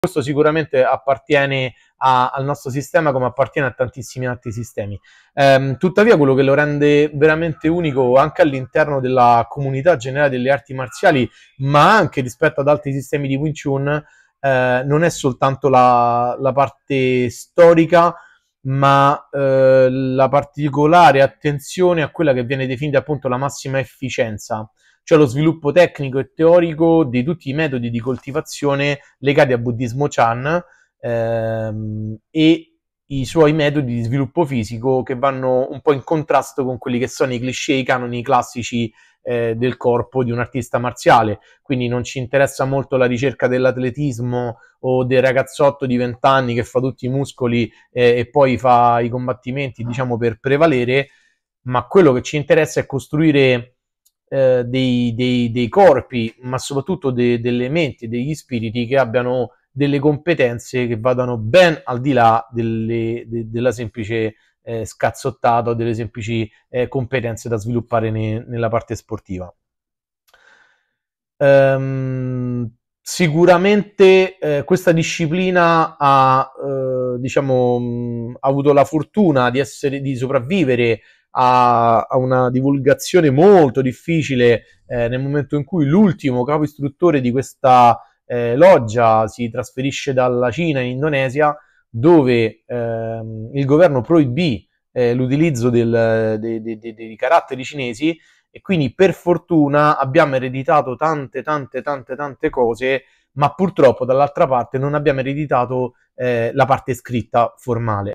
"L'Essenza del Black Flag Wing Chun"
Scopri i segreti del Black Flag Wing Chun: dove tradizione marziale incontra efficienza e disciplina interiore.